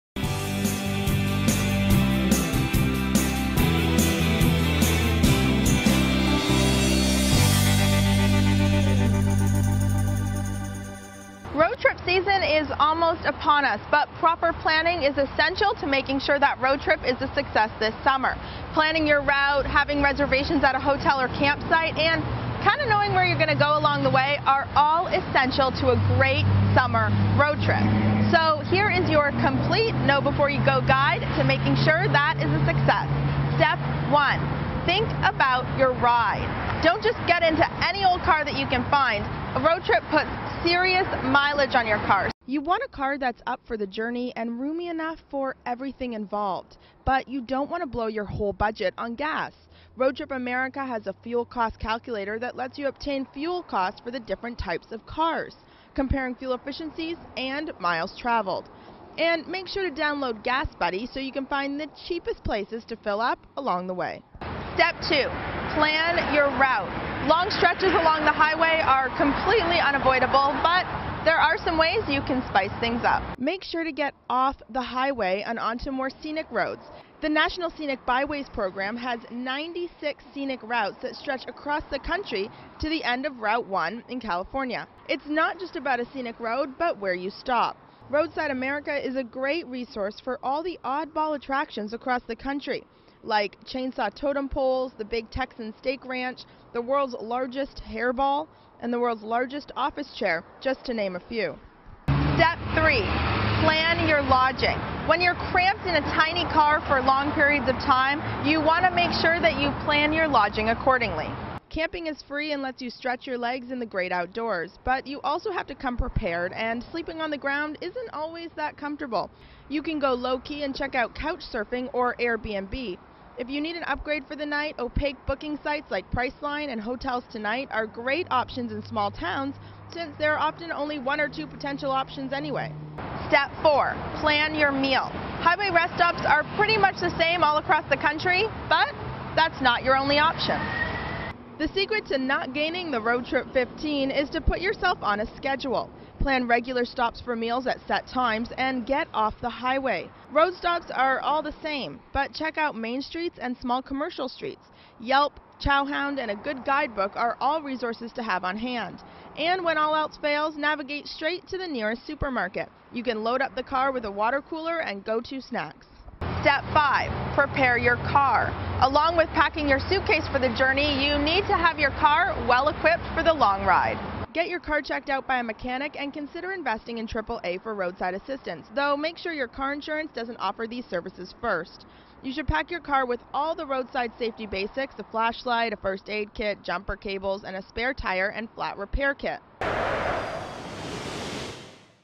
How to Plan a Road Trip
Road trip season is almost here, but proper preparation is the key part of any driving vacation. Before you set out, you need to plan a route, make reservations for where you want to stay and figure out the stops you want to make along the way. Travel Correspondent Alyssa Caverley has a complete Know Before You Go guide to a successful and fun road trip.